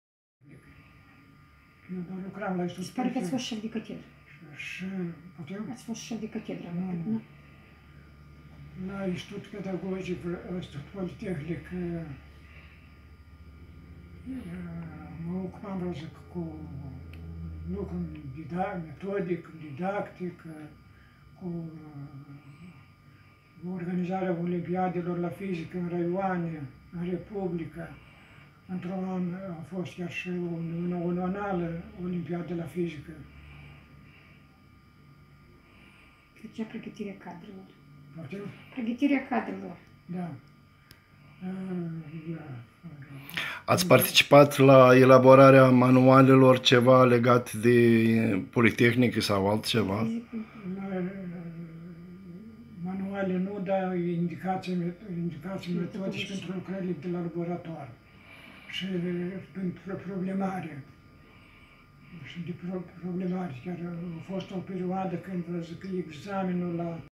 OAMENI ILUȘTRI AI SATULUI PIATRA: dr. VASILE ȘURA (4) Axenia Iasir Film
Axenia Iasir Film,  Savantul Vasile Șura  din s. Piatra, Orhei în dialog cu Axenia Iasir din s. Piatra, Orhei
Toate drepturile asupra acestui film( partea 1-4) apartin autoarei Axenia Iasir din Chișinău. Copierea sau folosirea acestui film fără permisiunea scrisă a autoarei este INTERZISĂ!